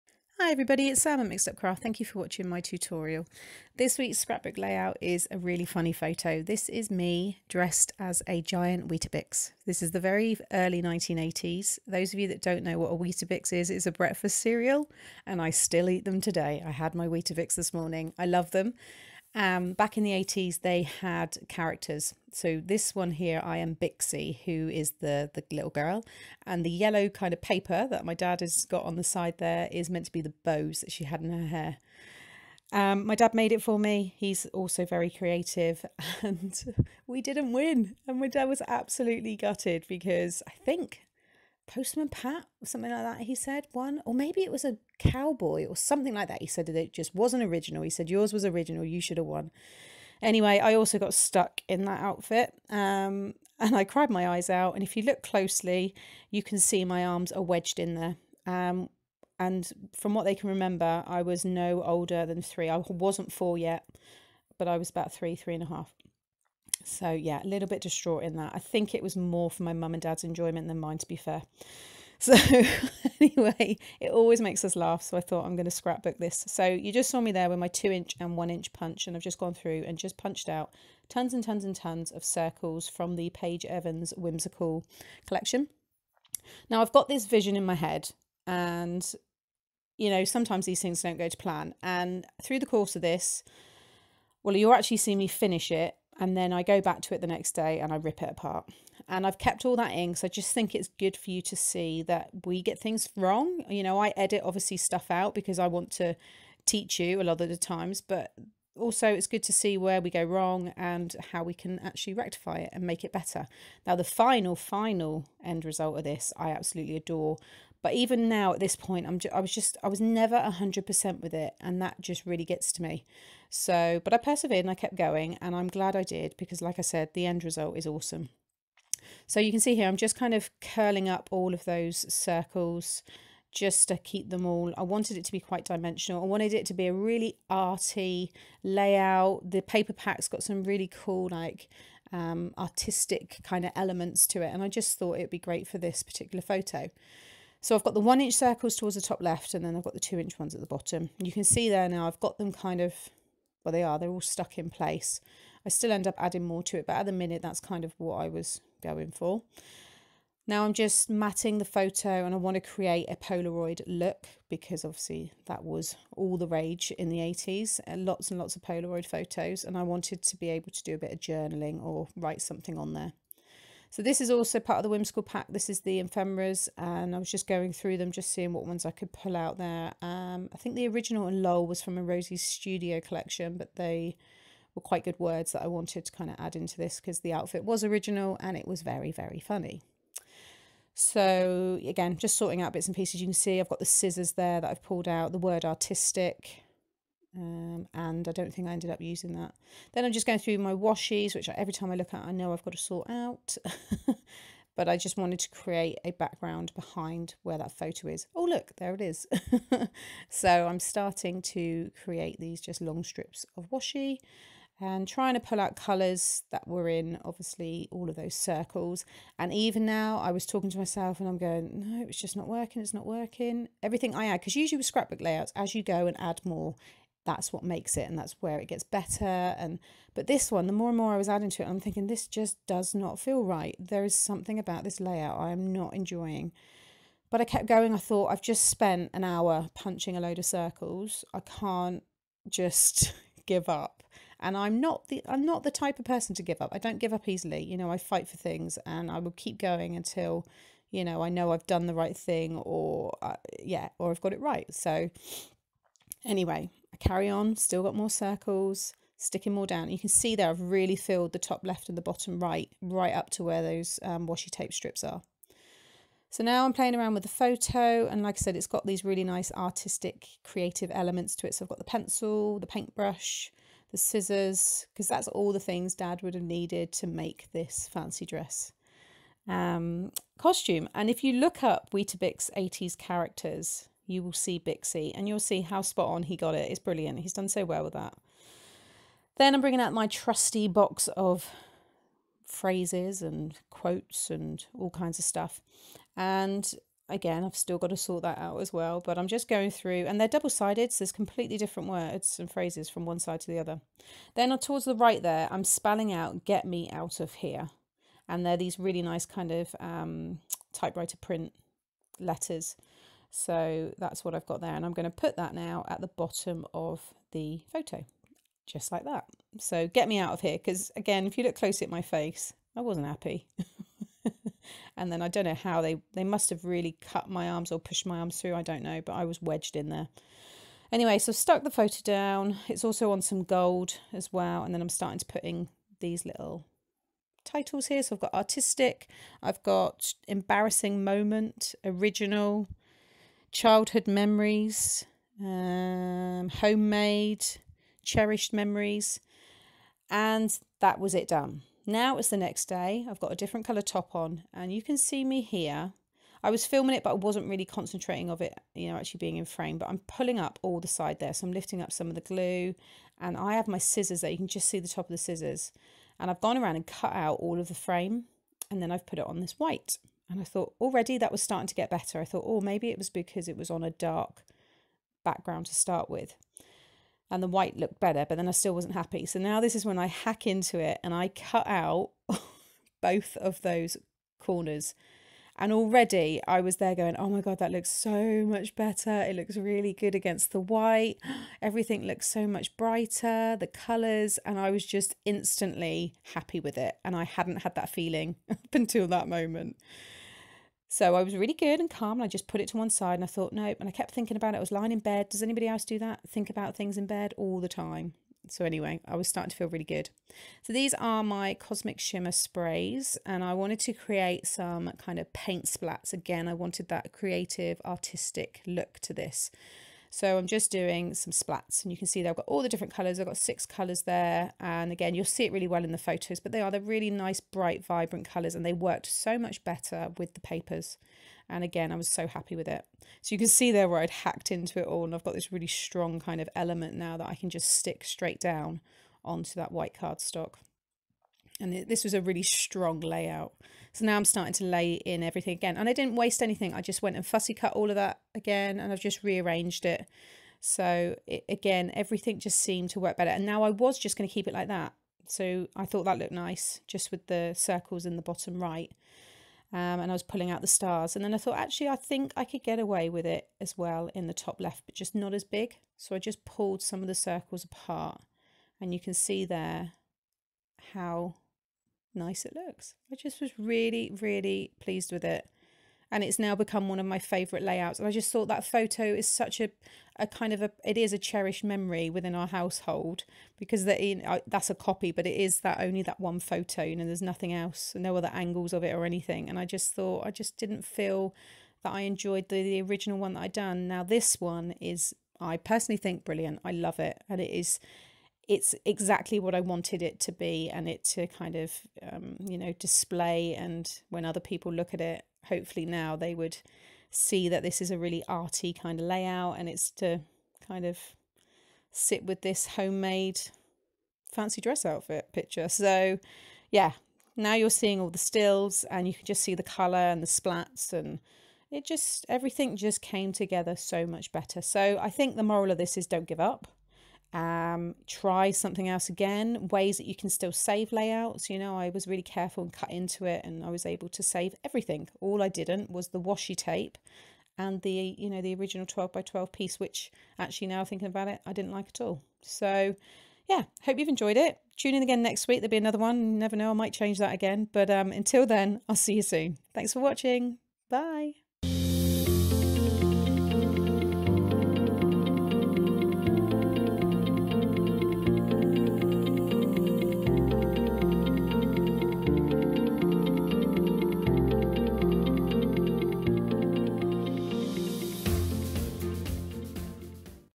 Scrapbook Layout "Artistic" | Process Video
In this tutorial I show you how to create a dimensional colourful scrapbook layout.

Sam